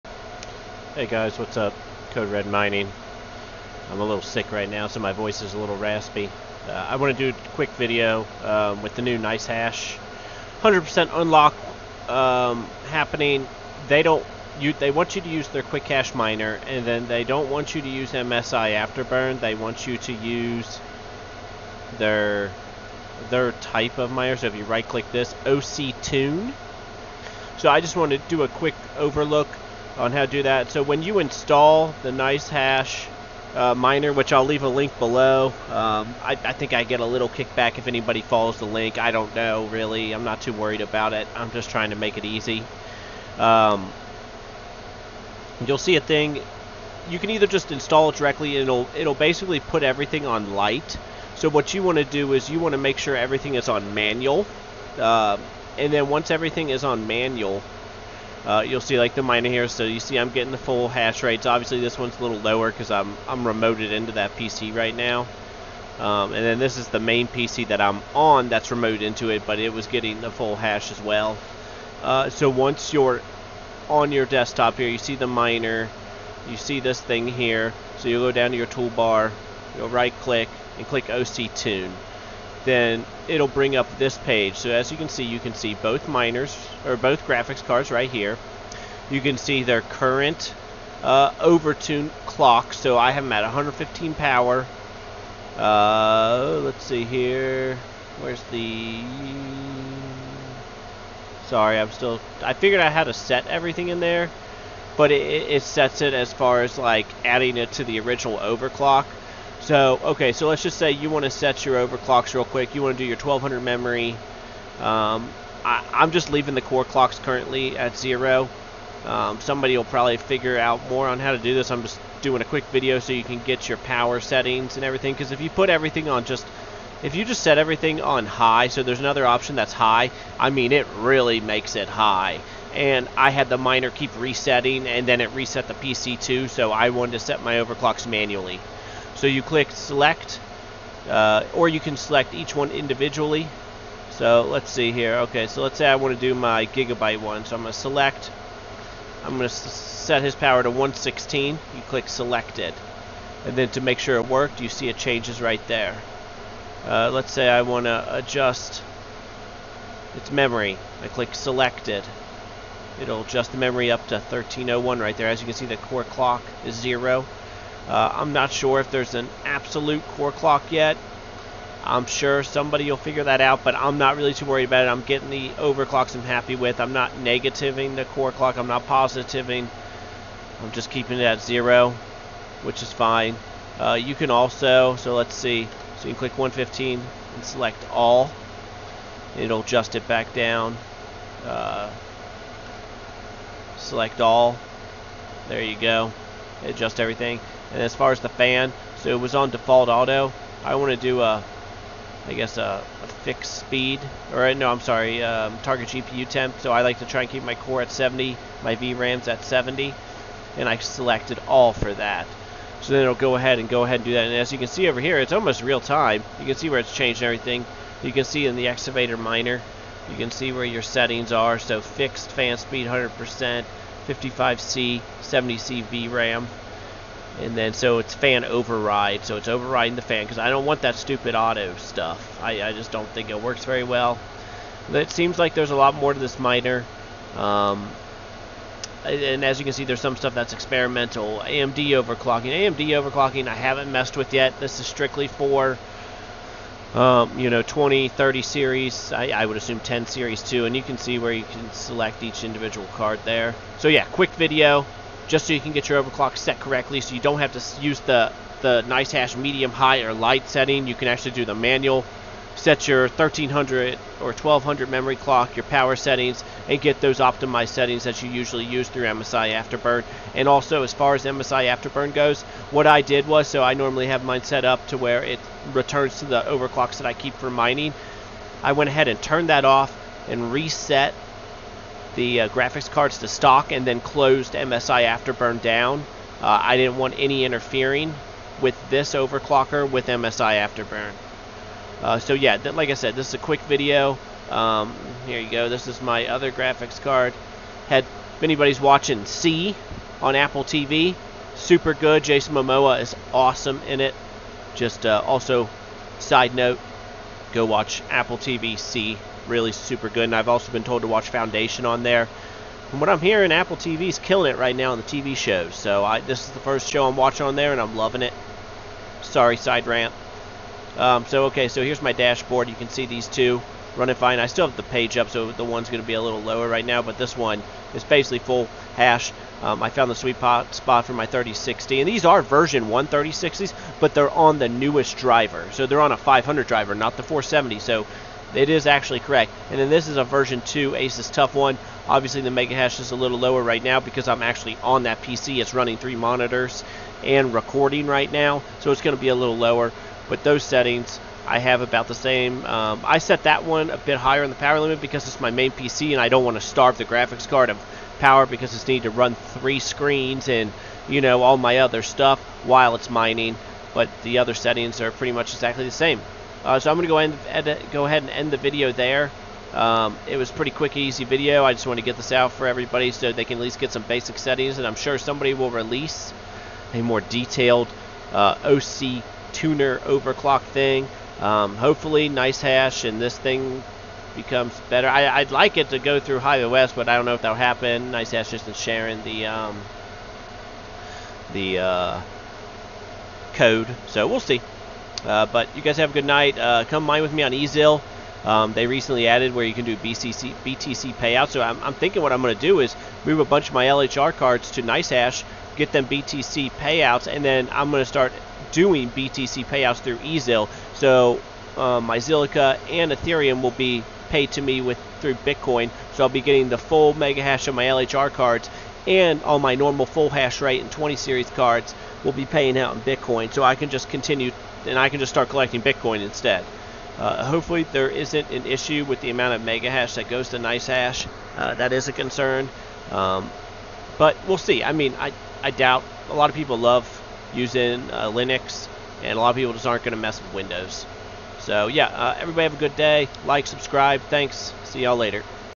NiceHash QuickMiner OCTune- 100% LHR Unlock
quick video showing how to us nice hash oc tune program so you can get good correct settings and dont have to use there basic settings. This is just for rtx cards as i have no amd cards that are lhr on windows.

Come Join HiveOS

BTC-  bc1q8jdvdnr3jlhnwsf9ez4cdulfues30lj90fvx3s
RTM- RNZg528M225RMNXVaYX8H6vaNKowHteYxn
ETH- 0x995B044d5Ffa17850dB5100505b59F7a1eE46da4
RVN- RMN1dsUAhqQpGCFMq5hj47vXZ28YUSqGom
XMR-49RnshoH9X1DuSkTvwX5W48EvKrENjkdmVDqo8ehcsoCHjKdwjTaaMxiE6P43MngNSFdStz9Lb1chXdhjmmWUpry4stQF3m
Flux- t1XA5zqfCPX4St1DC3oM8KPhHriYRRYPpH4
ERGO- 9h3pPhpSktFjzuhSebcgaUCVwRFy26n4SdFuQ7wuziNjAeQ5TDn